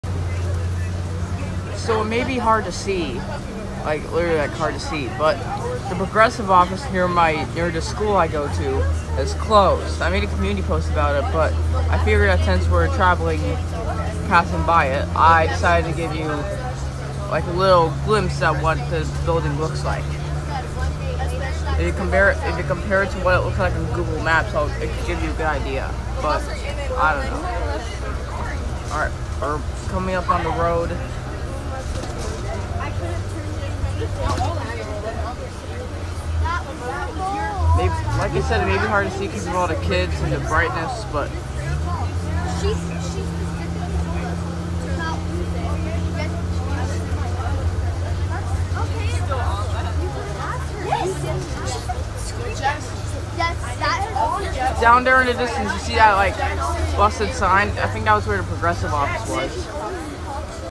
What P-P-Progressive looks like now
So remember how I made a community post where the P-P-Progressive office near my school closed? Well, I’m going to give you a tiiiiiiny look at what it looks like now.